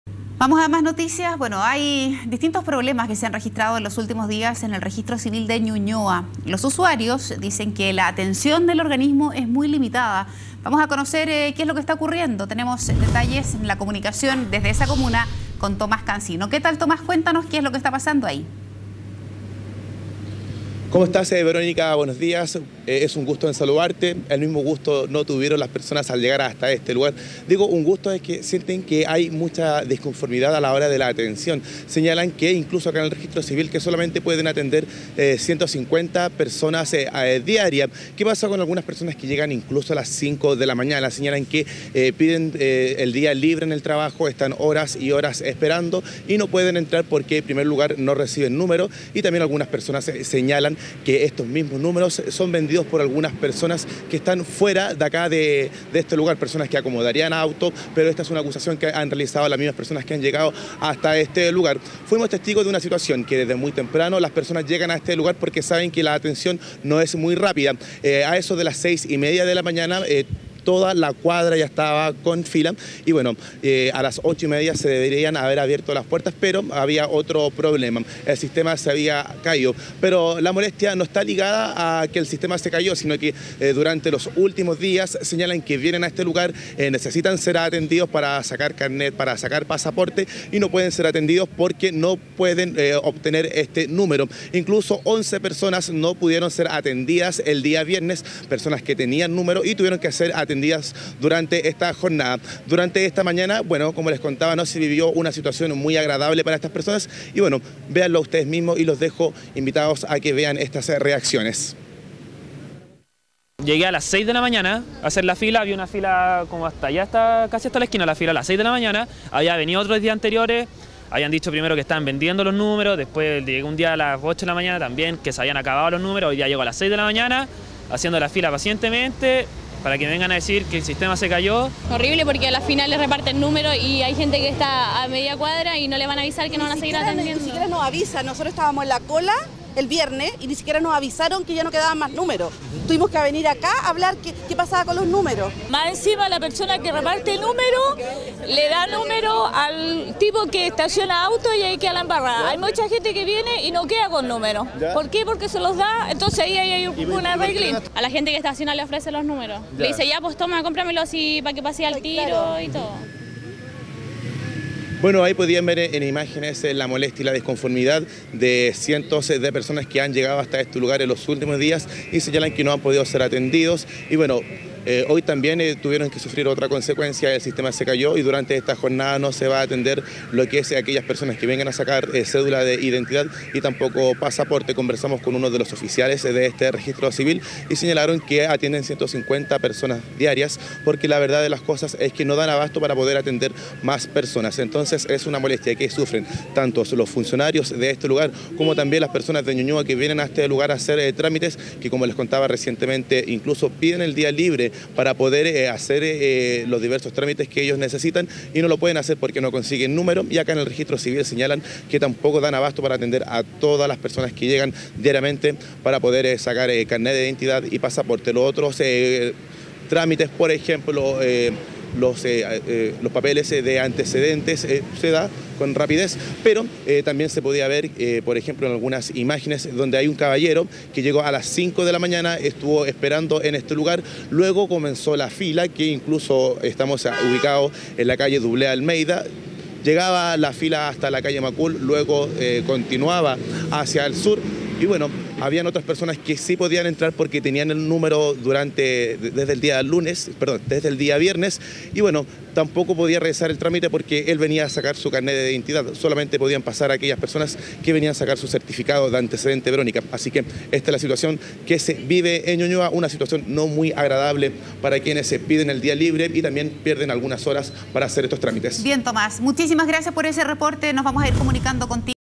Usuarios denuncian fallas en el Registro Civil del Gran Santiago
Diversas irregularidades son las que mantienen el descontento de la gente, principalmente por los largos tiempos de espera para realizar los trámites.